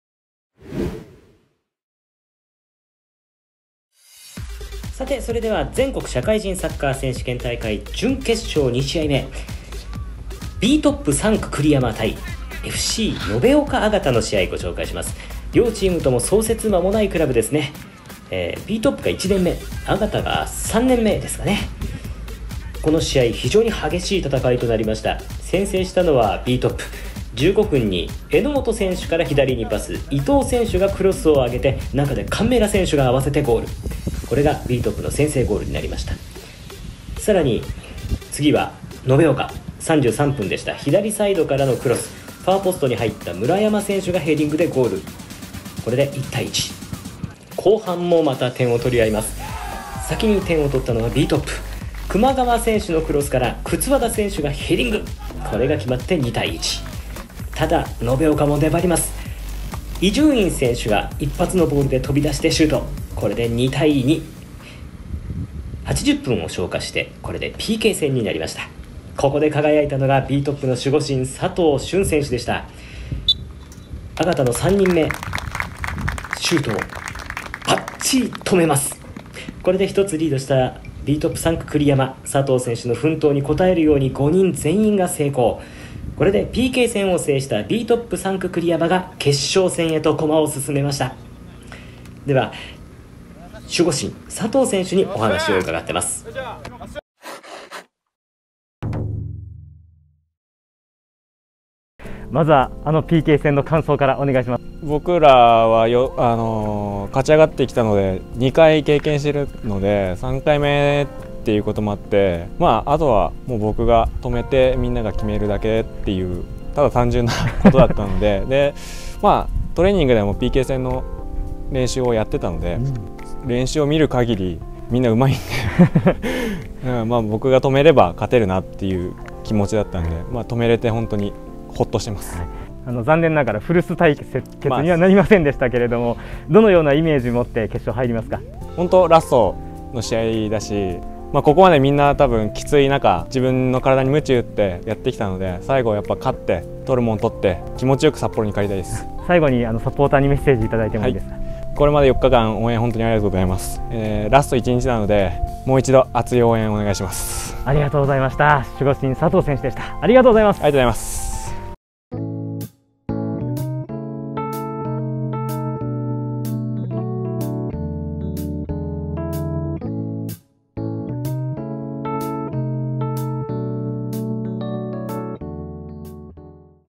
最速J昇格を目指す「創設1年目」BTOPサンクくりやま、全社決勝進出！GK佐藤隼が“僕が止めれば勝てる”と思ったワケとは
10月18日に行われた全国社会人サッカー選手権大会準決勝、BTOPサンクくりやま対FC延岡AGATAの試合を取材してきました！